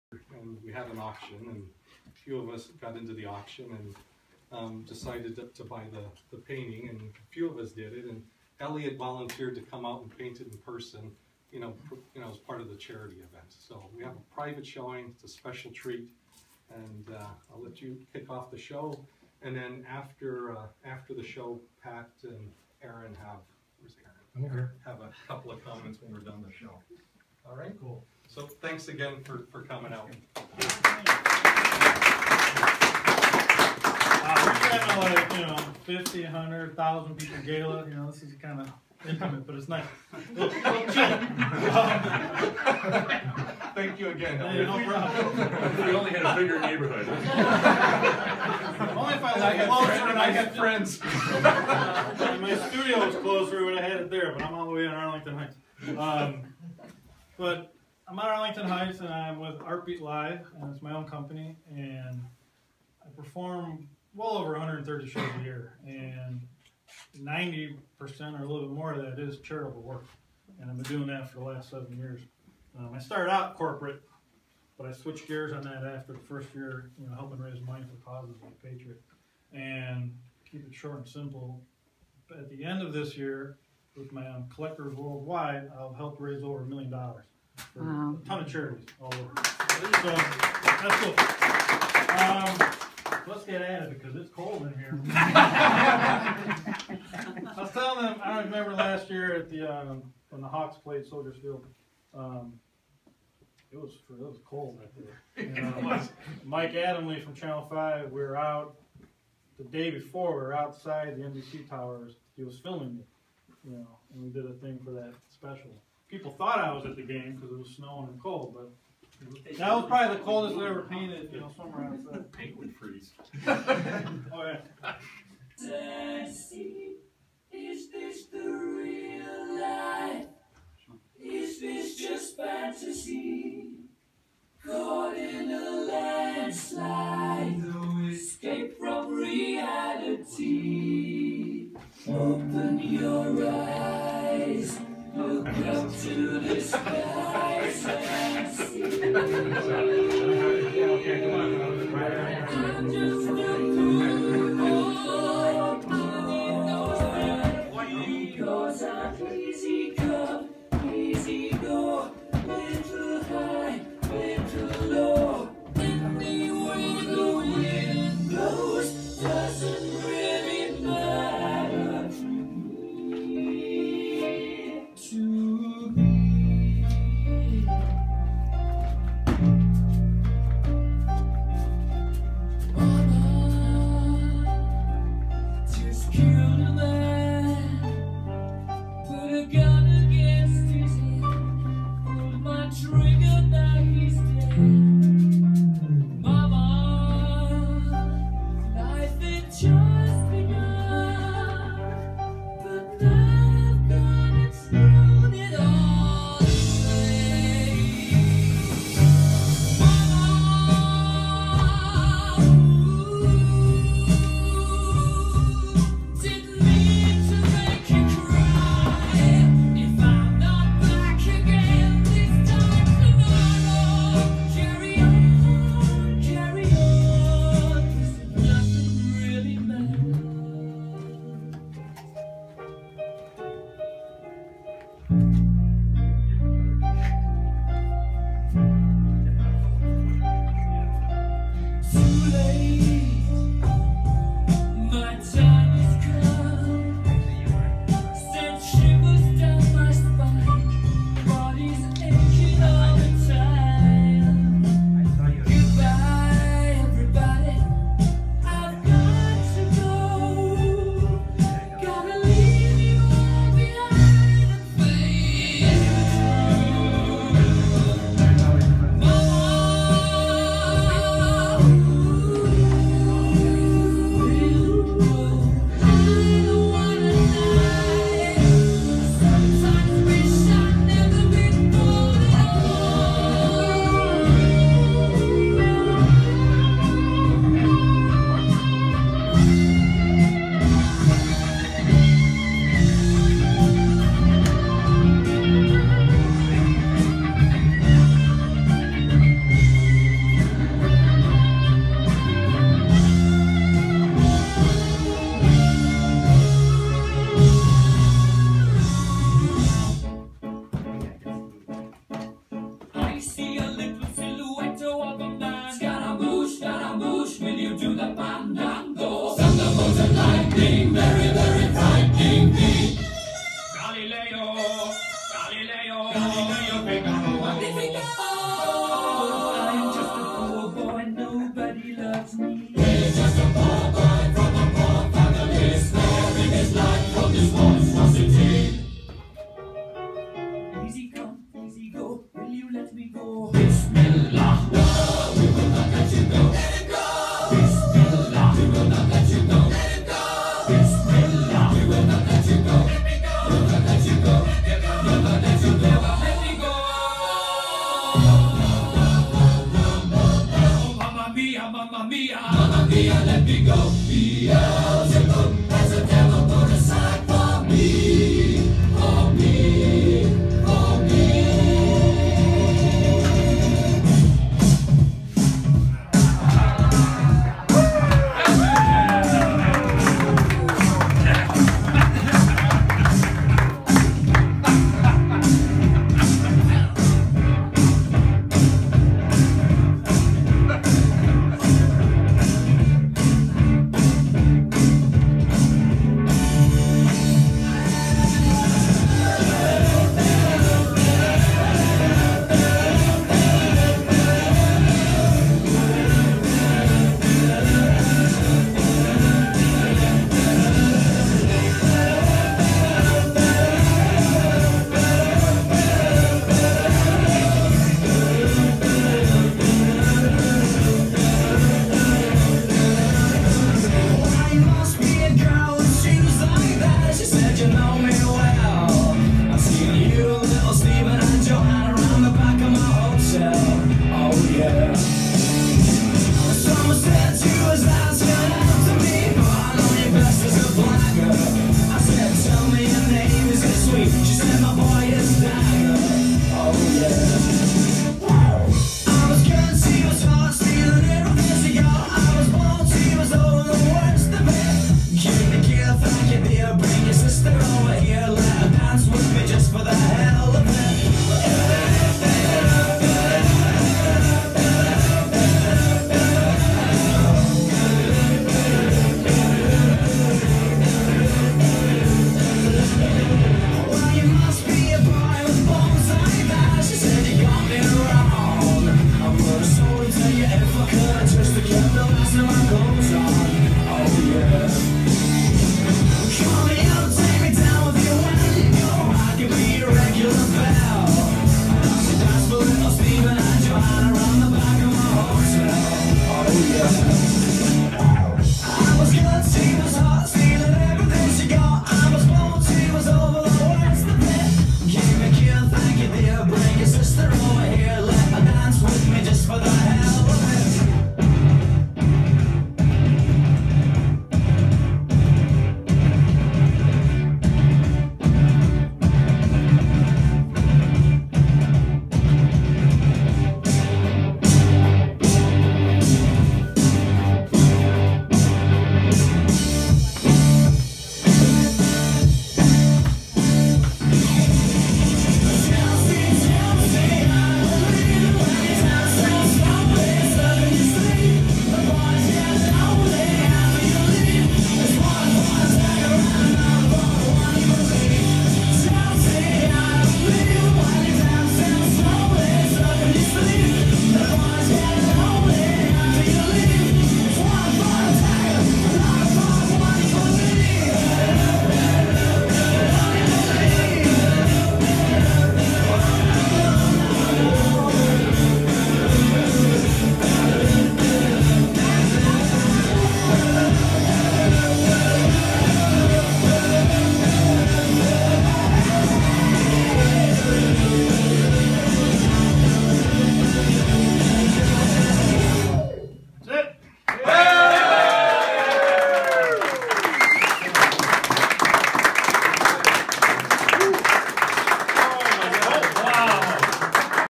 Blackhawks painting - DPF Fundraiser 2014
Disabled Patriot Fund event by performance artist Elliott From, Artbeat Live.